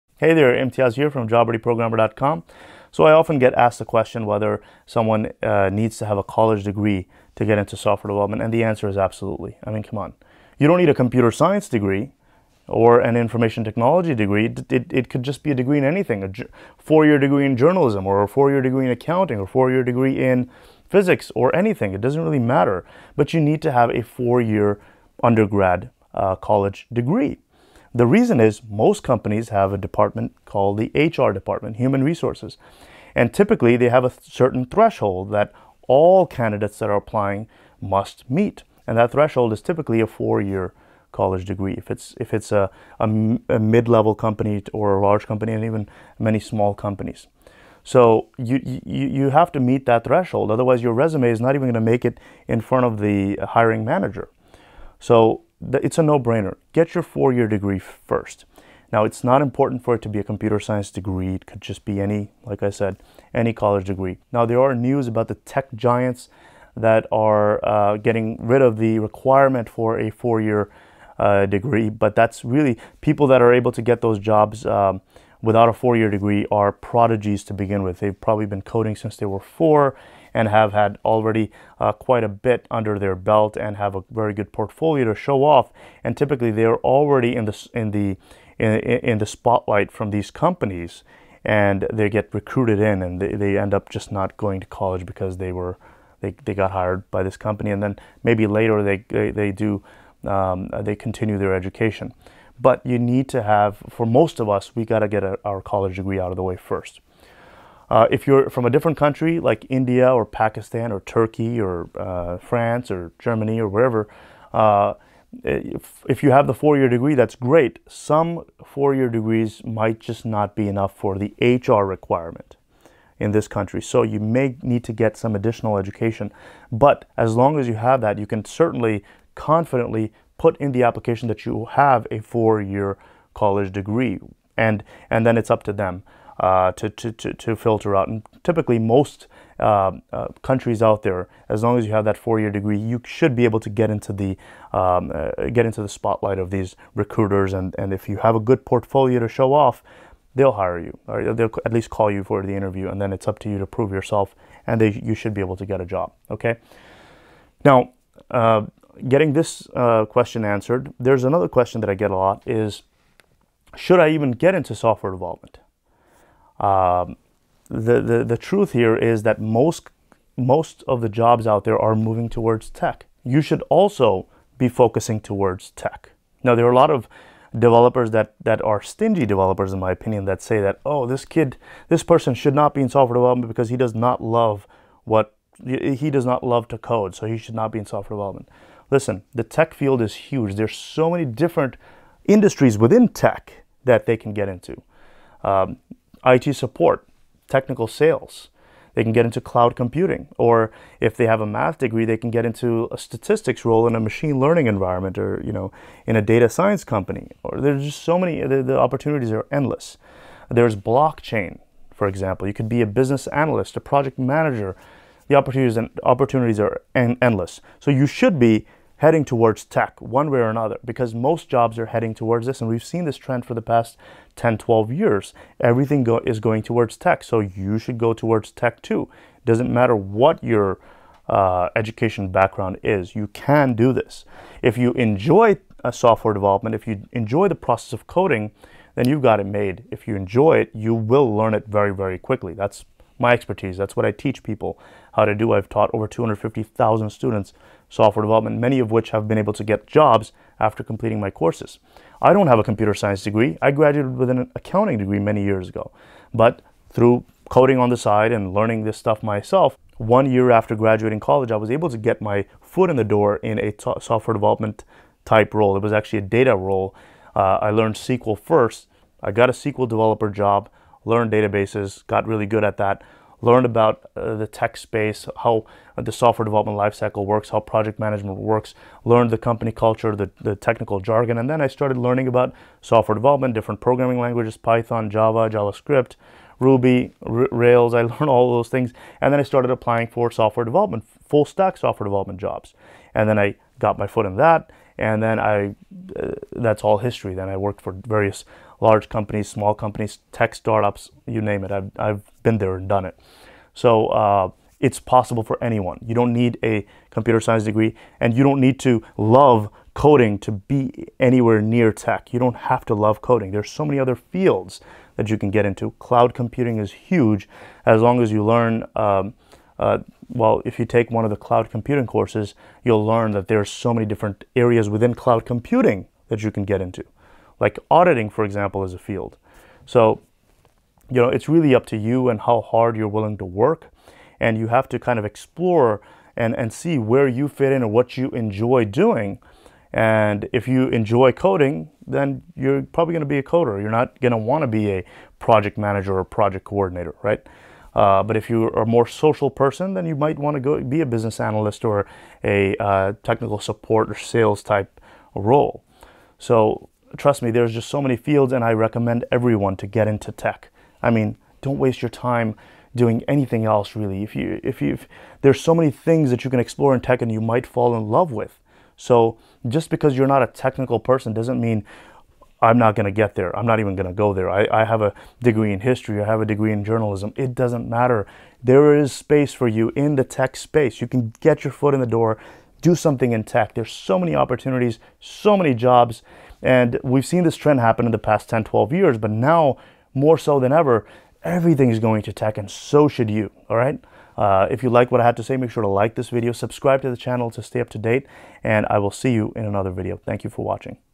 Do you Need a College Degree for Software Development or Tech?
In this video, I explain whether you need a college degree to get into software development or tech!

- Access all of my software development courses using the discount
➡️ Timestamps:
0:00 Intro
0:28 Reason for a four-year degree requirement
1:04 News from tech giants on “no degree required”
1:42 Note on variations in certain countries
2:37 Career in Tech
4:02 Personal experience in Tech
5:13 Career opportunities in Tech
6:34 Outro